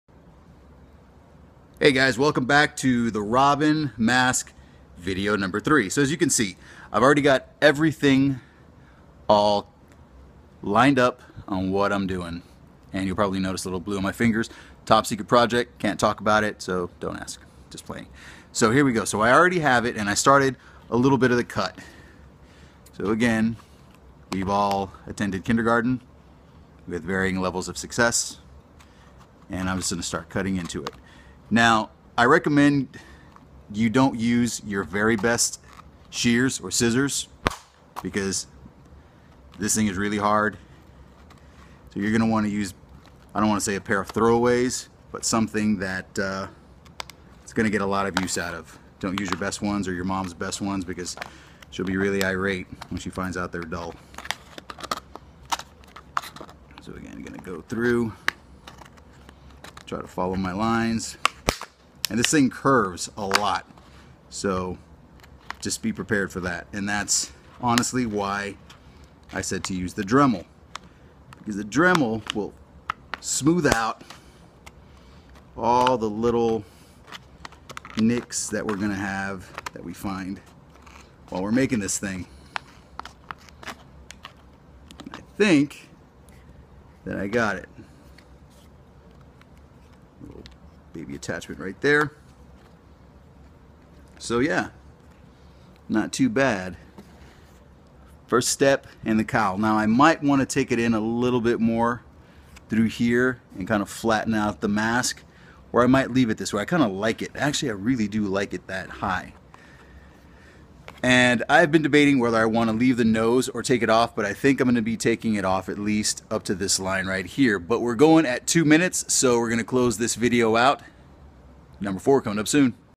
Robin mask tutorial 3
Continuing to convert a Batman DOJ mask into a Robin mask